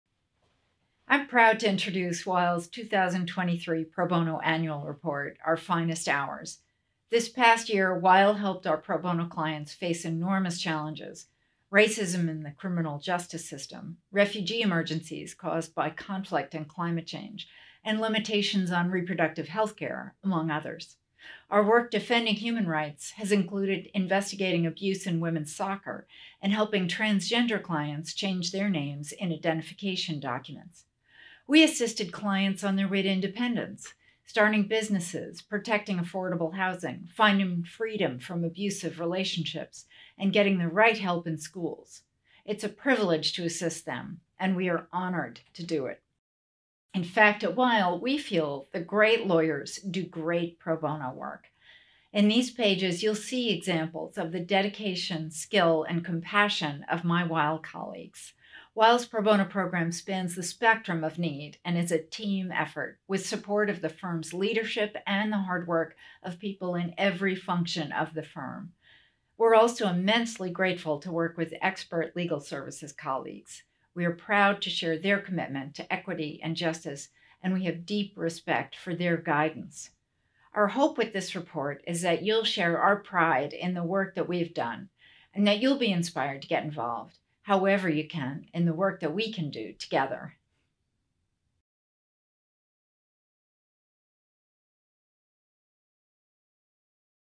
Weil's Pro Bono Annual 2023 Introduction with Miriam Buhl
Pro Bono Counsel Miriam Buhl talks about the report and its many examples of Weil’s efforts over the past year to further racial justice, help refugees, protect human rights, and serve clients across the spectrum of need.